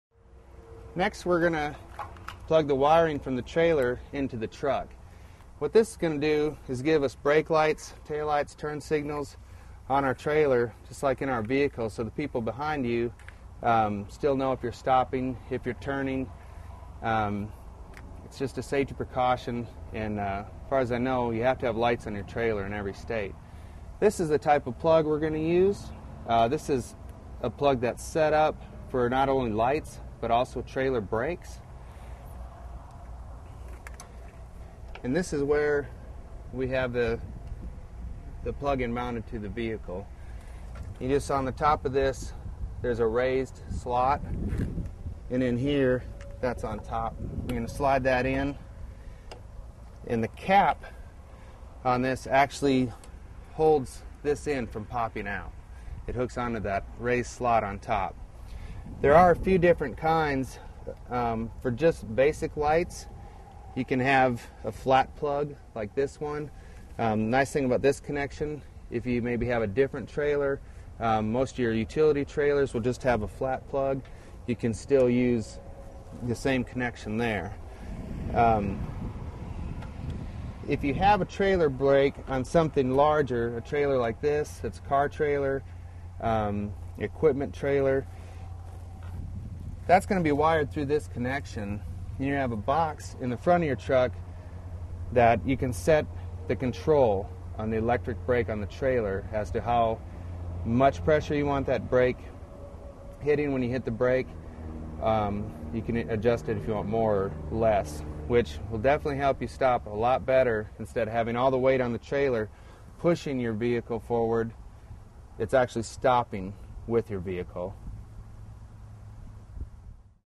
Connecting Truck Trailer Light Wiring
Connecting Truck Trailer Light Wiring. Part of the series: Connecting & Loading Truck Trailers. Wiring from a trailer to a truck provides brake and tail lights. Connect truck trailer electrical wiring with tips from a custom car repairman in this free auto maintenance video.